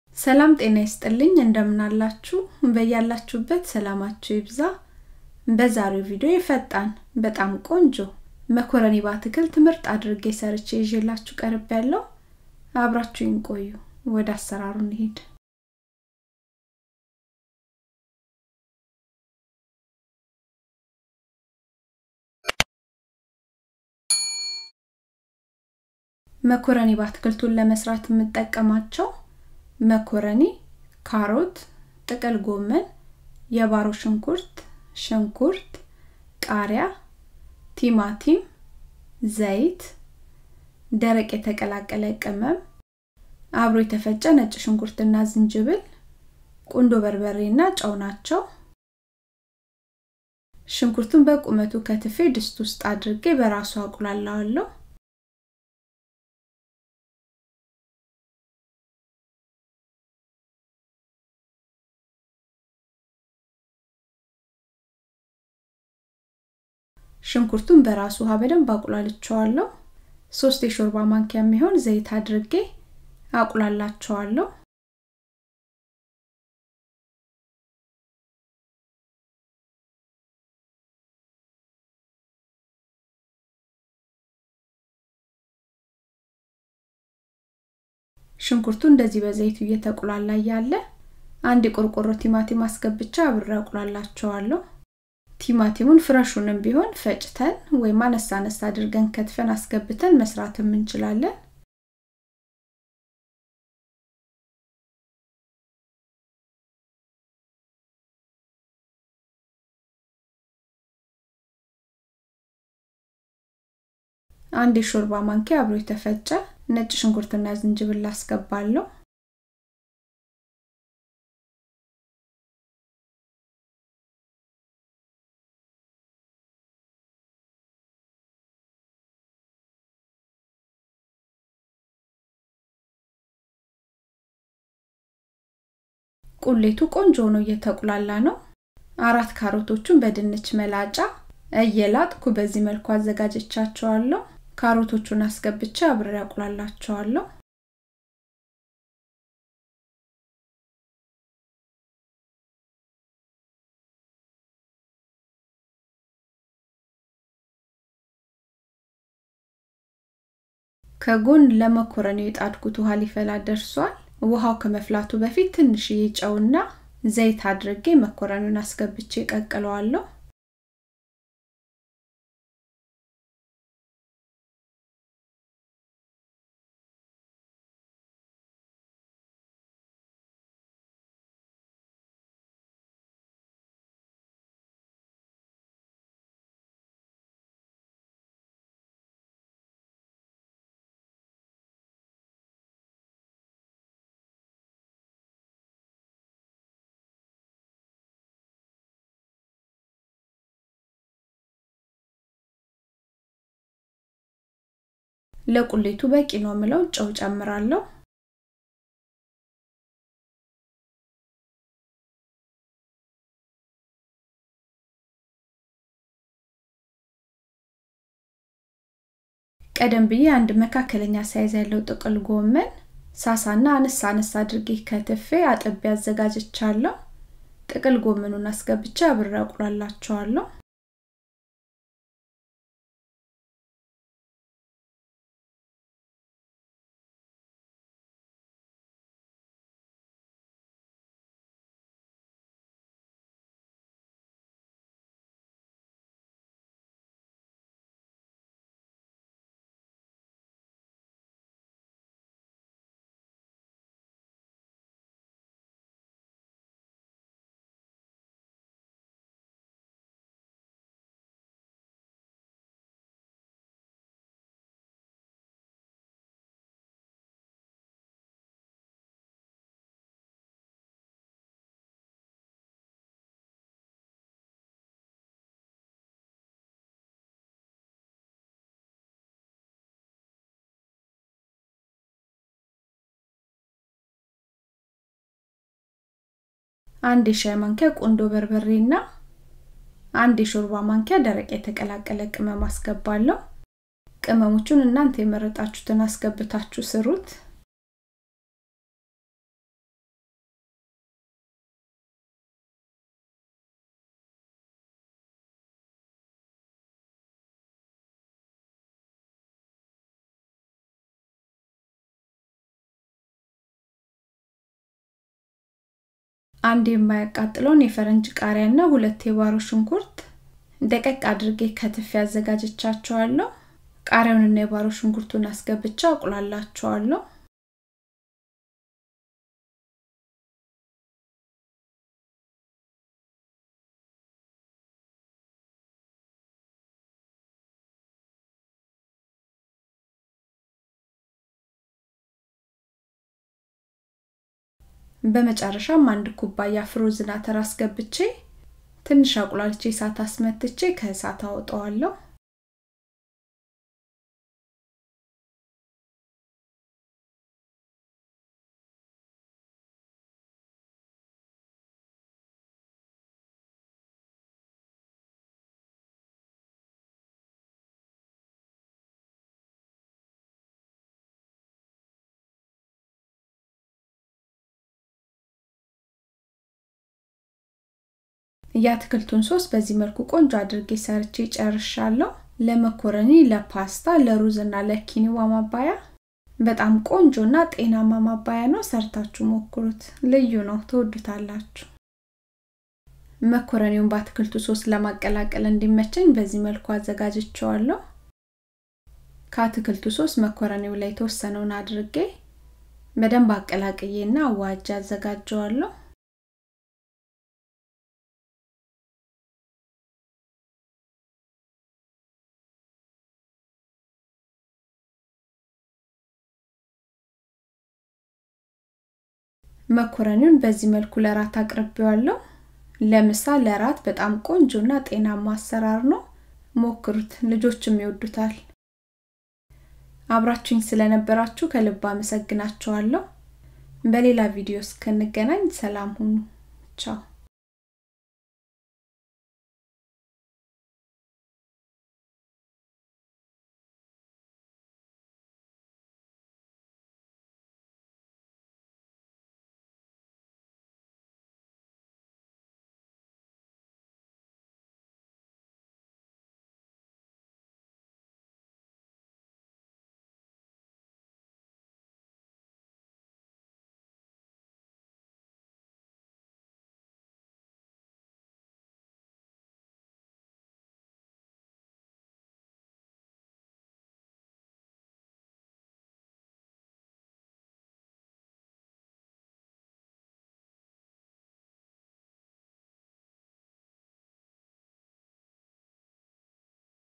ፈጣን ጤናማ መኮረኒ በአትክልት አሰራር
መኮረኒውን ለመስራት የተጠቀምኩት
* መኮረኒ
* ሽንኩርት
* አብሮ የተፈጨ ነጭሽንኩርትና ዝንጅብል
* ካሮት
* ጥቅልጎመን
* ቲማቲም
* ቃሪያ
* የባሮ ሽንኩርት
* ዘይት
* ደረቅ ቅመም
* ቁንዶ በርበሬ
* ጨው